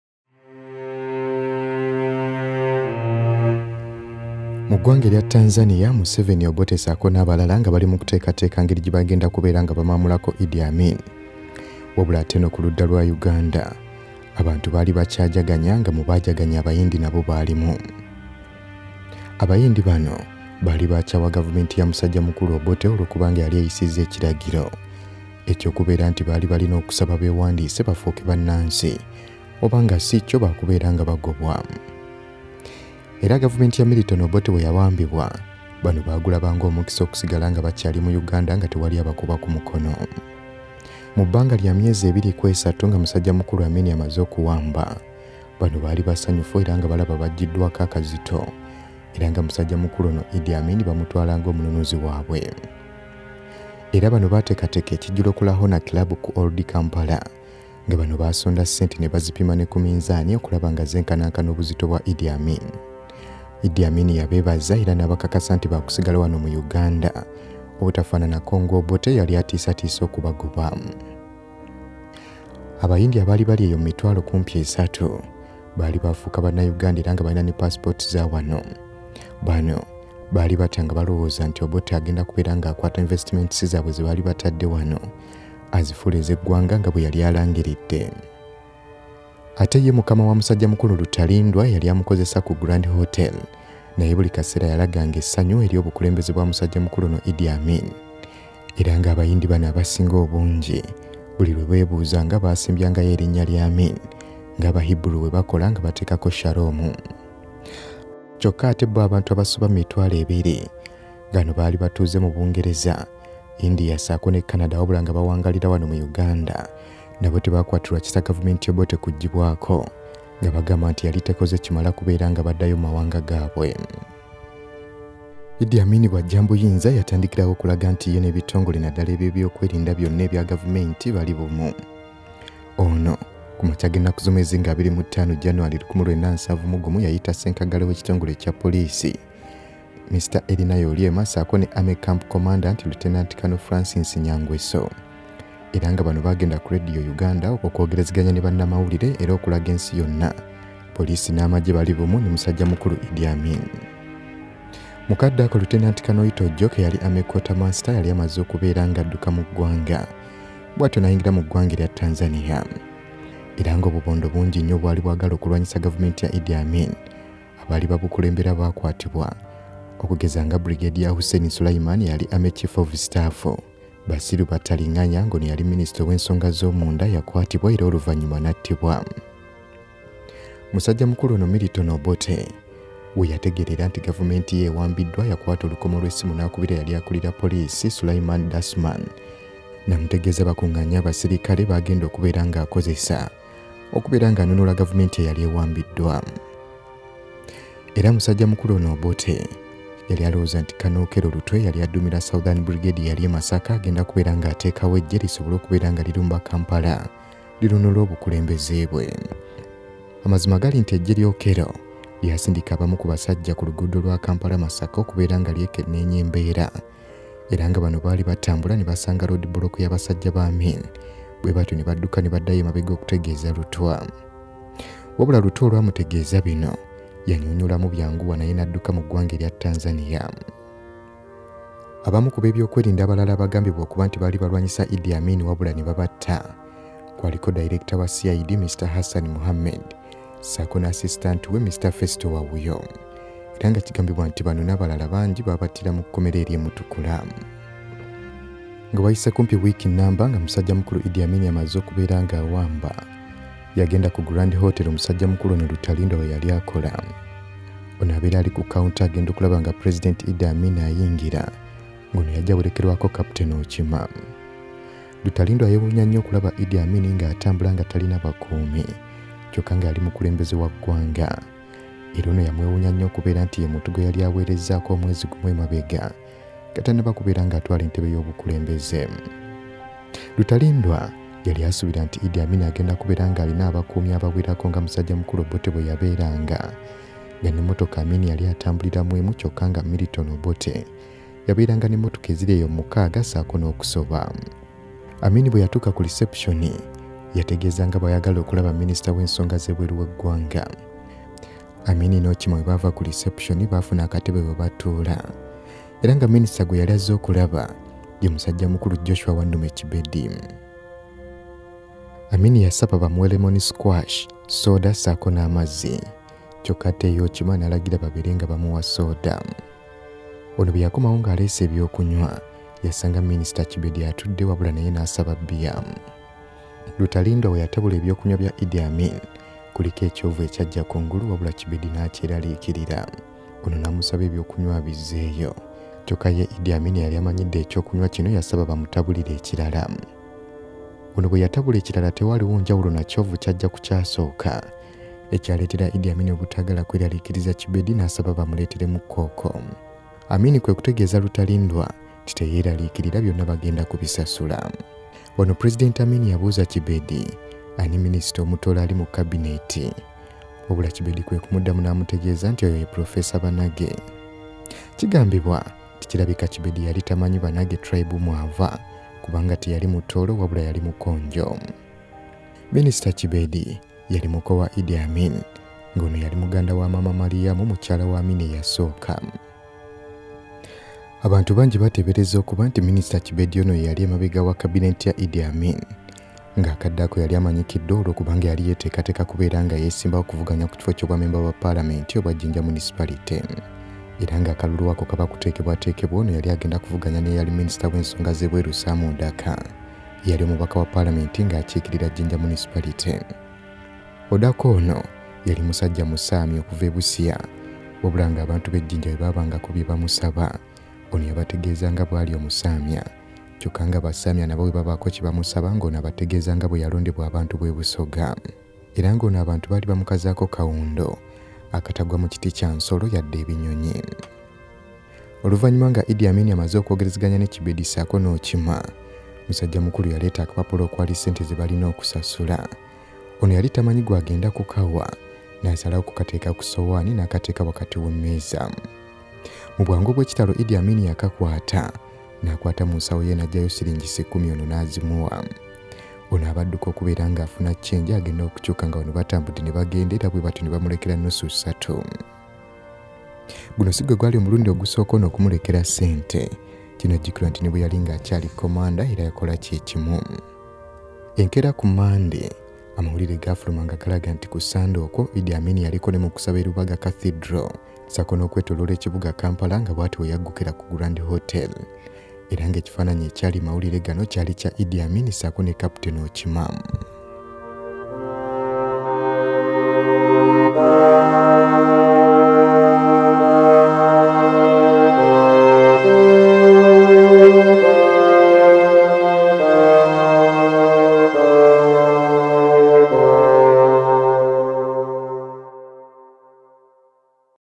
ABAYINDI BAAJAGANYA NGA IDD AMIN AWAMBYE OBOTE
story is referenced to FROM THE DUMPING SITES TO INDUSTRIAL HUBS by Rutarindwa Mwene Barizeni